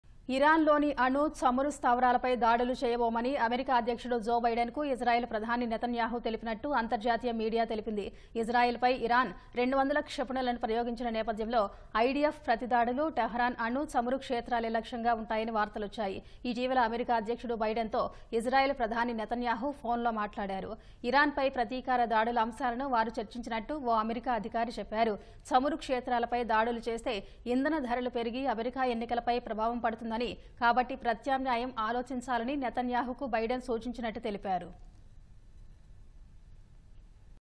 Netanyahu Tells U.S. That Israel Will Strike Iranian Military, Not Nuclear Or Oil, Targets
ఇరాన్ లోని అణు, చమురు స్థావరాలపై దాడులు చేయబోమని అమెరికా అధ్యక్షుడు జో బైడెన్ కు ఇజ్రాయెల్ ప్రధాని నెతన్యాహు తెలిపినట్లు అంతర్జాతీయ మీడియా తెలిపింది. ఇజ్రాయెల్ పై ఇరాన్ 200 క్షిపణులను ప్రయోగించిన నేపథ్యంలో.. ఐడీఎఫ్ ప్రతిదాడులు టెహ్రాన్ అణు, చమురు క్షేత్రాలే లక్ష్యంగా ఉంటాయని వార్తలువచ్చాయి. ఇటీవల అమెరికా అధ్యక్షుడు బైడెన్ తో ఇజ్రాయెల్ ప్రధాని నెతన్యాహు ఫోన్ లో మాట్లాడారు. ఇరాన్ పై ప్రతీకార దాడుల అంశాలను వారు చర్చించినట్లు ఓ అమెరికా అధికారి చెప్పారు. చమురు క్షేత్రాలపై దాడులు చేస్తే ఇంధన ధరలు పెరిగి అమెరికా ఎన్నికలపై ప్రభావం పడుతుందనీ. కాబట్టి ప్రత్యామ్నాయం ఆలోచించాలని నెతన్యాహుకు బైడెన్ సూచించినట్లు తెలిపారు